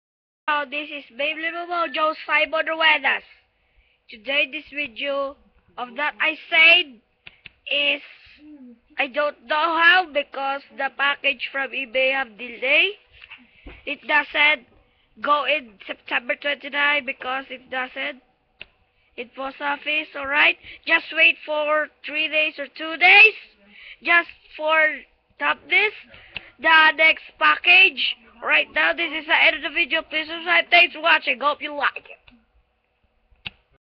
Beyblade delay ebay beyblade pacage :(
-Sorry for the delay of the beyblade pacage because  the sender says that they're will be delayed-